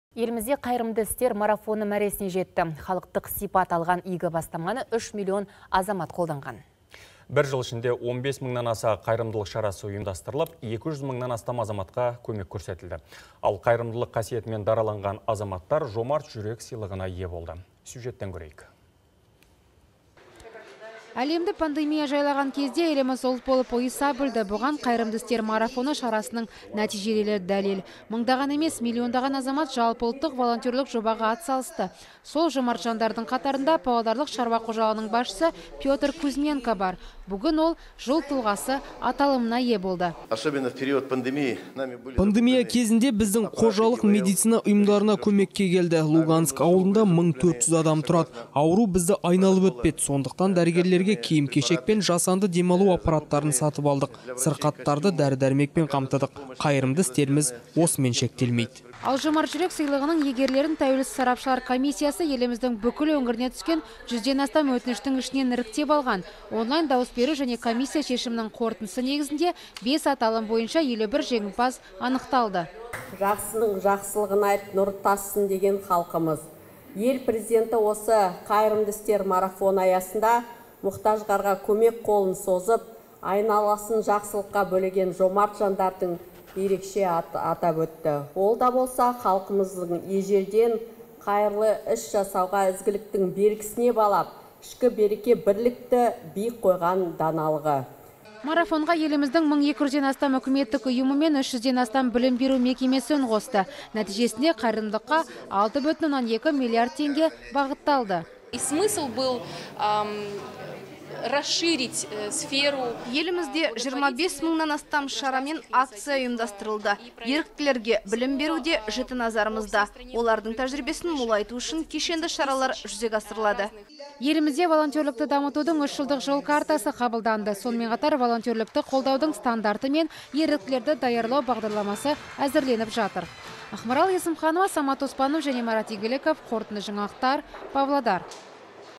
Қайырымды жандарға «Жомарт жүрек» сыйлығы табысталды
Елімізде «Қайырымды істер марафоны» мәресіне жетті. Халықтық сипат алған игі бастаманы үш миллион азамат қолдаған. Бір жыл ішінде 15 мыңнан аса қайырымдылық шарасы ұйымдастырылып, 200 мыңнан астам азаматқа көмек берілді. Ал халқымыздың қанына сіңген қайырымдылық қасиетімен дараланған азаматтар «Жомарт жүрек» сыйлығына ие болды.